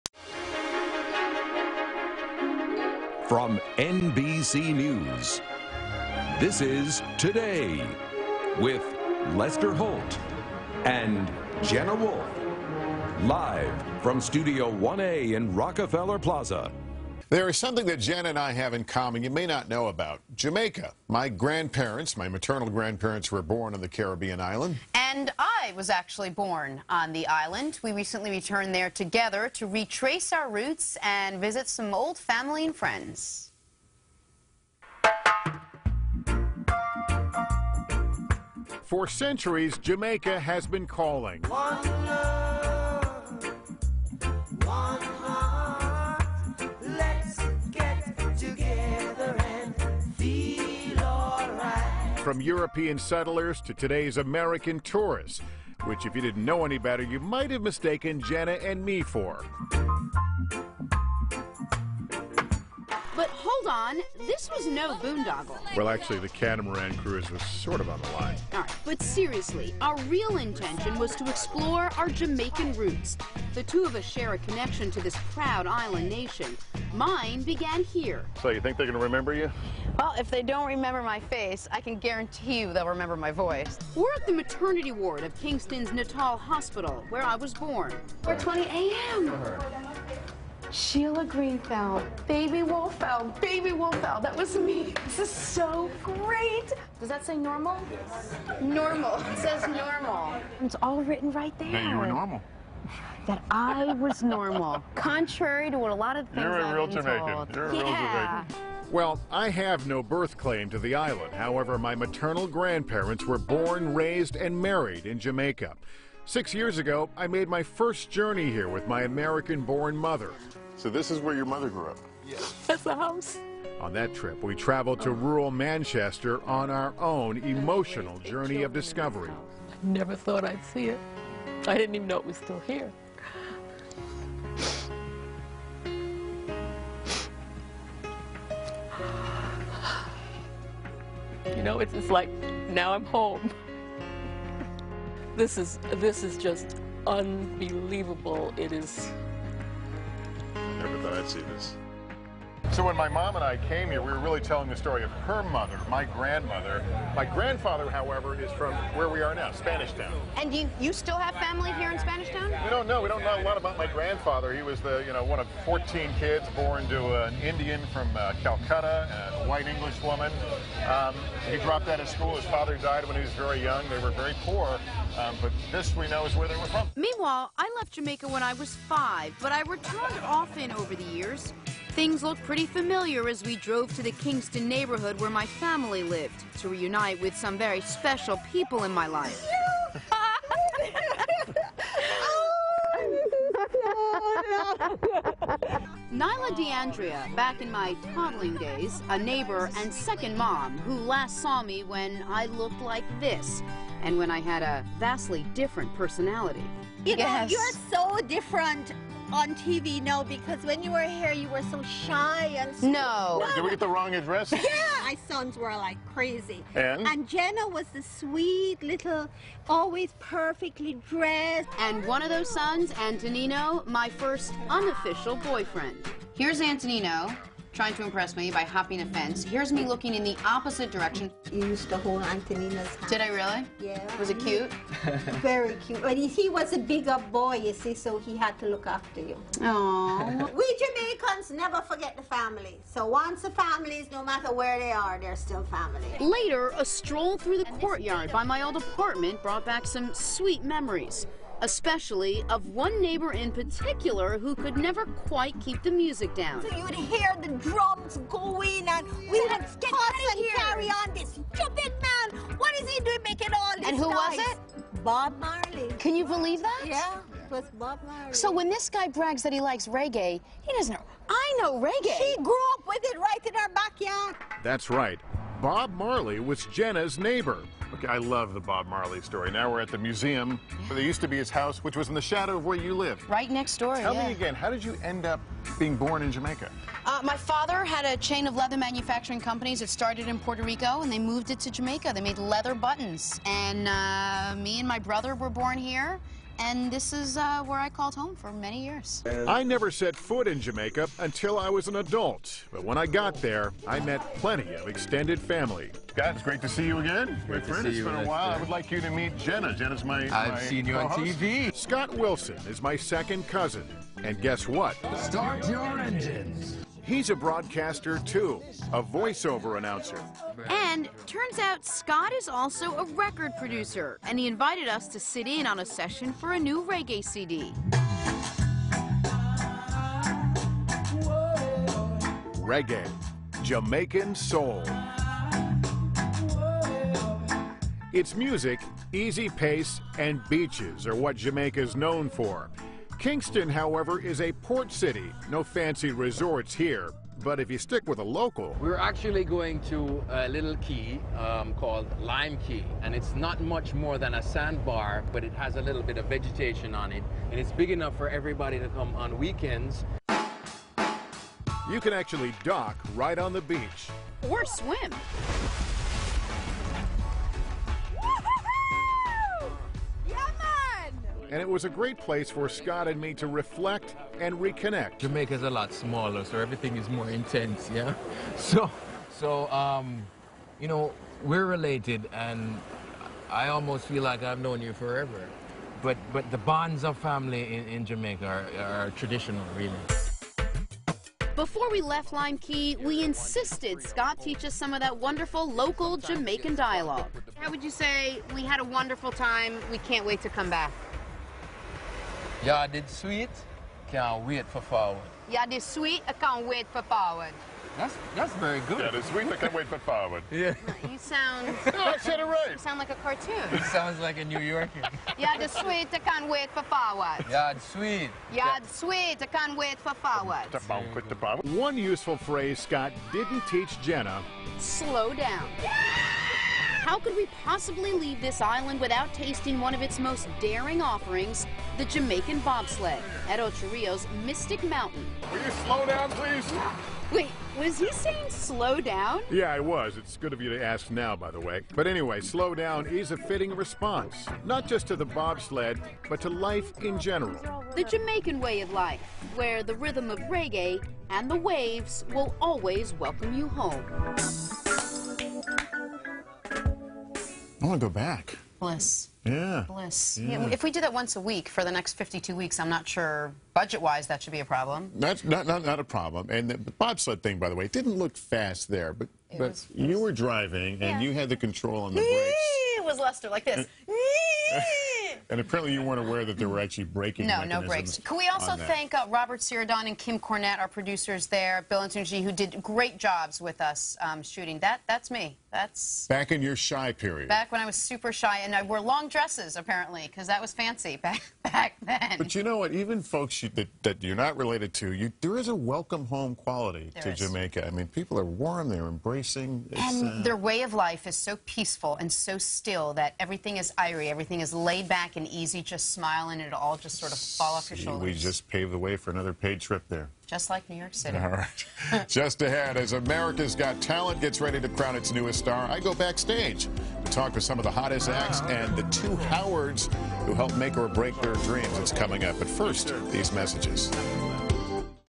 NBC NEWS HOSTS Visit Jamaica
They explore their Jamaican heritage